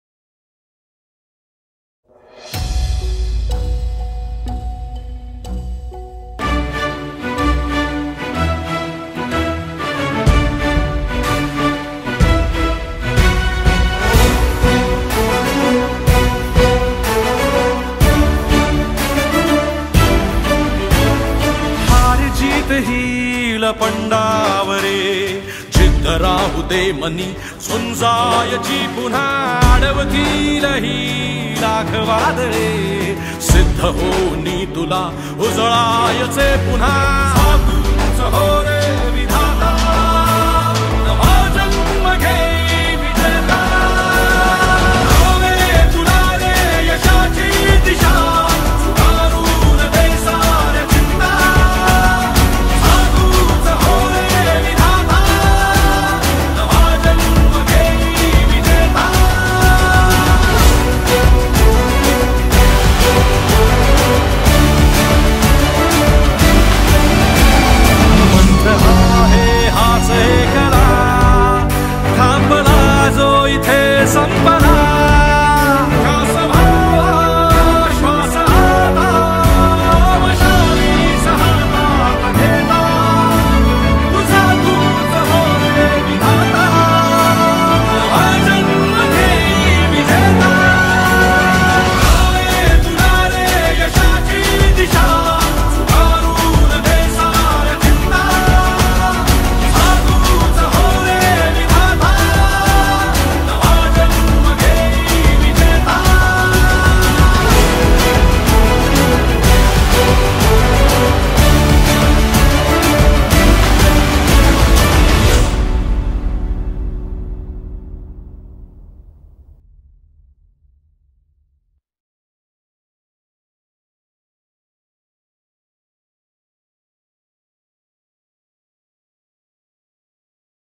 एखाद्या मंत्र्यांचा दौरा फिका पडेल असं शक्ती प्रदर्शन, ग्रामपंचायत निवडणूक रॅली, श्री अजय चौधरी ✌🏻
Sakurdi, pune , grampanchayat nivadnuk, साकुर्डी